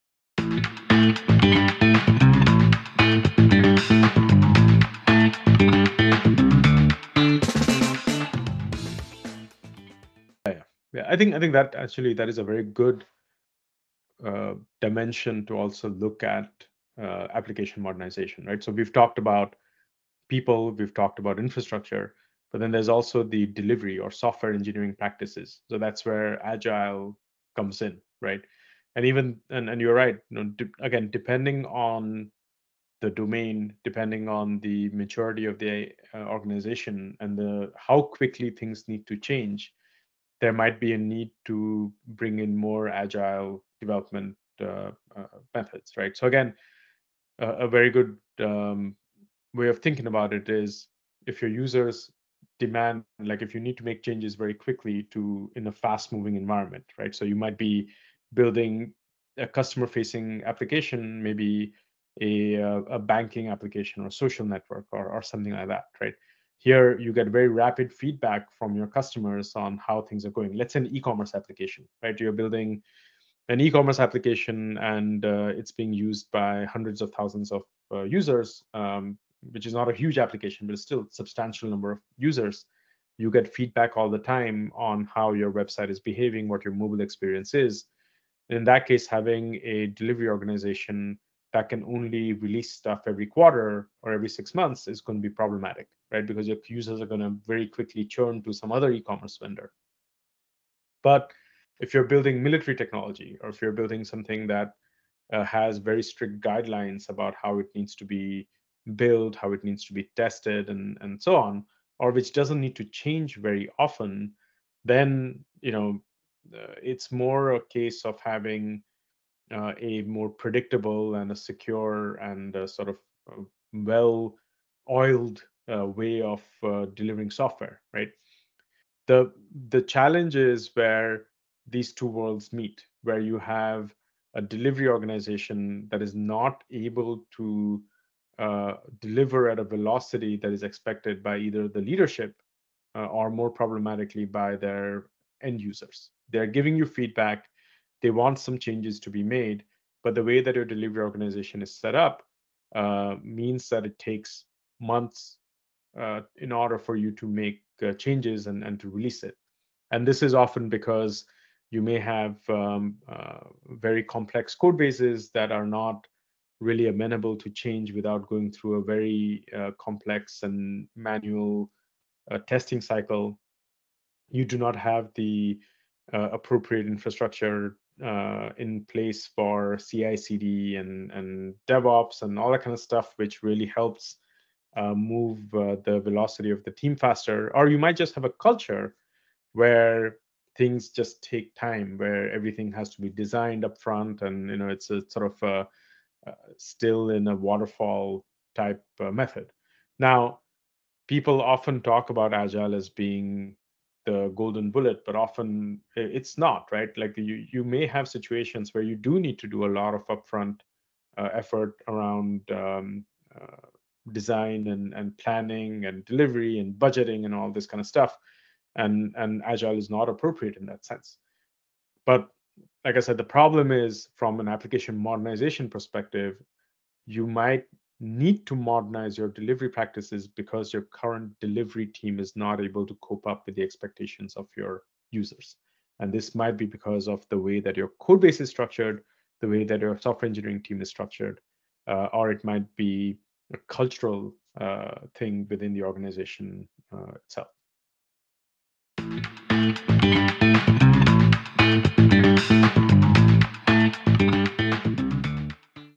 Software Engineering Processes of Application Modernization
Watch the TeXploration episode on the critical balance needed in software delivery across various industries. We explore how organizations can integrate agile methodologies to enhance responsiveness without compromising the structured processes required in more regulated environments.

*Key Takeaways:*
1. The importance of agile methodologies in environments demanding quick adaptability.
2. Challenges faced by sectors that require rigorous standards, such as military technology.
3. Strategies for merging agile flexibility with structured delivery protocols.
4. Insights on overcoming cultural and technical barriers to implement CI/CD and DevOps.